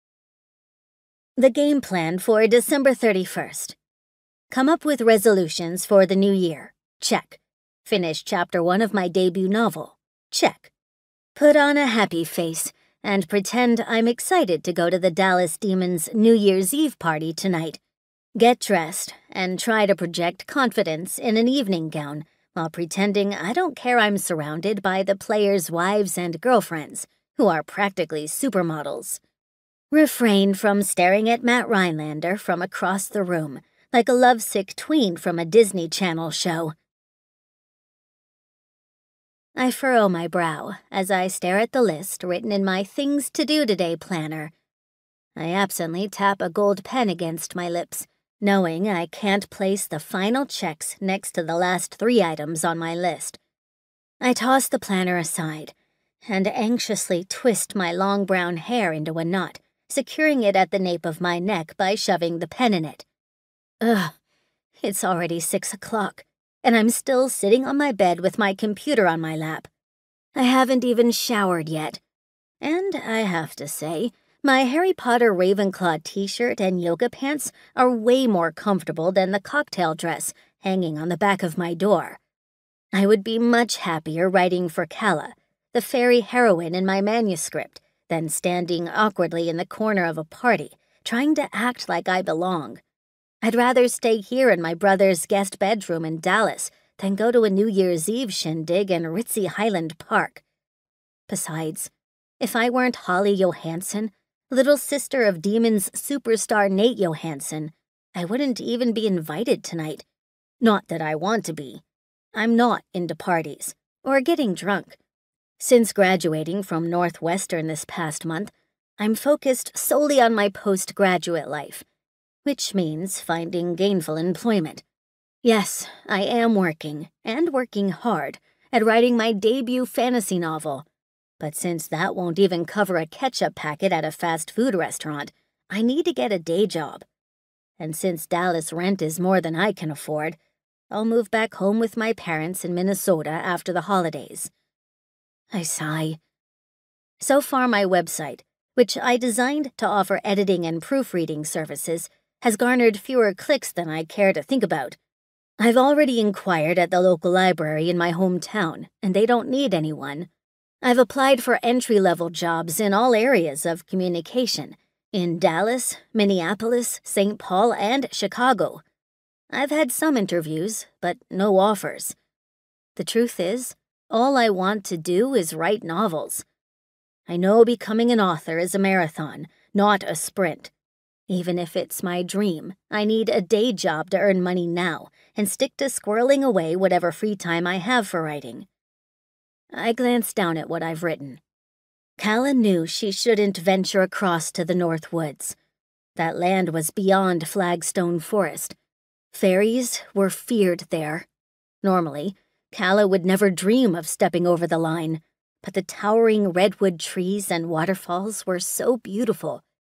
Audiobook Sample: On Thin Ice
Holly Johansson knows she's living in a fantasy world, but she's trying to be realistic. Like, she knows becoming a bestselling author isn't going to happen overnight, so she will need to get a day job now that she has graduated from college. Just like she knows her brother's best friend, hockey superstar Matt Rhinelander, would never, ever return her feelings.

How could he?

She'd rather stay home with her nose in a book, while he has a reputation as a barhopping ladies' man. So, while she may be hopelessly in love with him, she knows better than to expect that he could ever be anything more than her brother's thoughtful friend who actually listens when she talks.

Or could he?

After Matt comes to Holly's rescue, she wonders if he might have deeper feelings, too. When Matt confirms he sees her as more than his teammate's baby sister, will it put them both on thin ice?